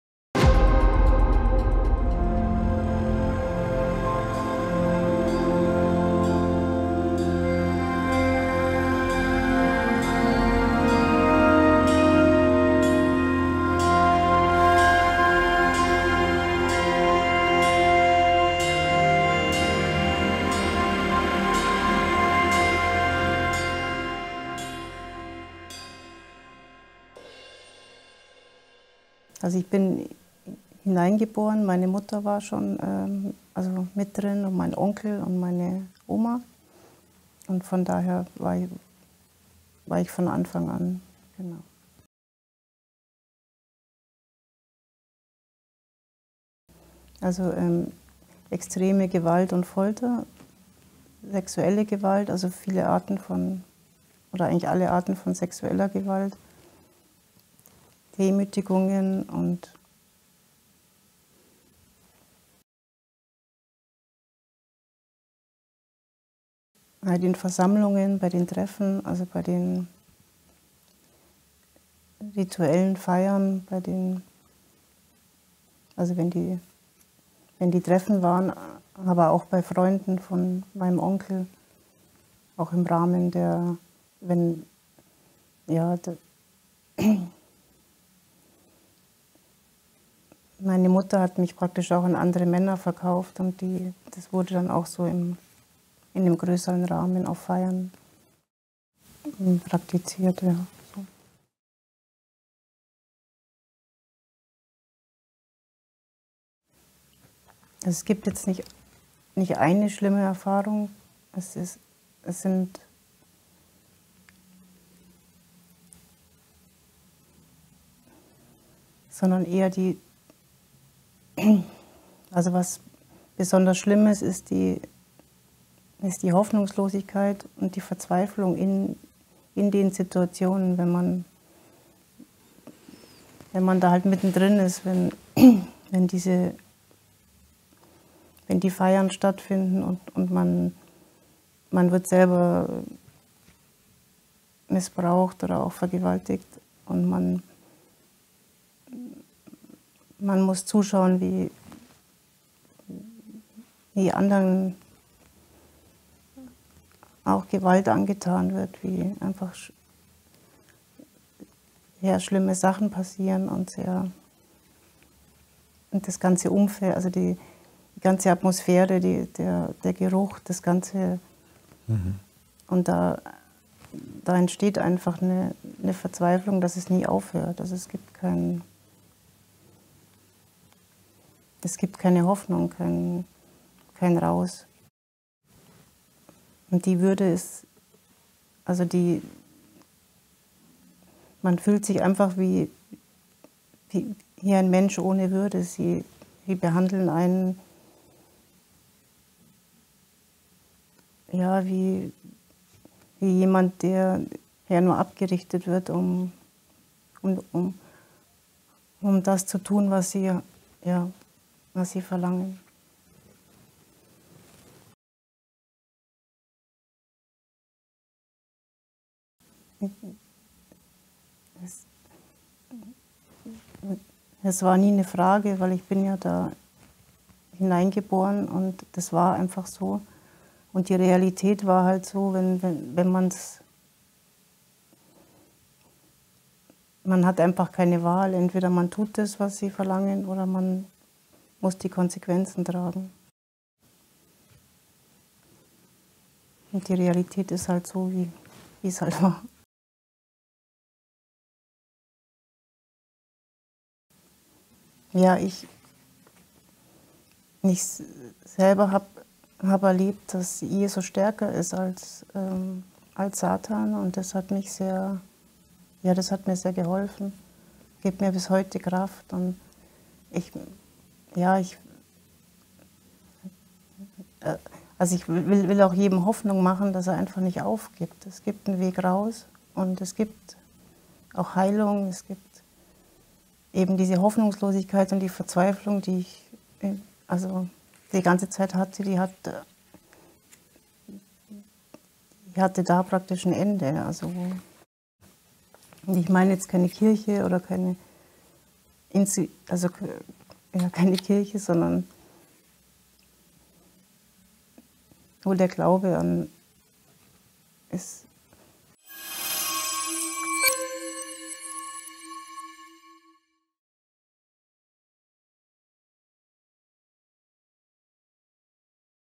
CLAUDIA | TESTIMONY
Claudia (45) experienced ritual abuse from the age of 0 to 18 – in North Rhine-Westphalia, Bavaria, Austria, Belgium, and the Netherlands. "I was born into the cult," she says.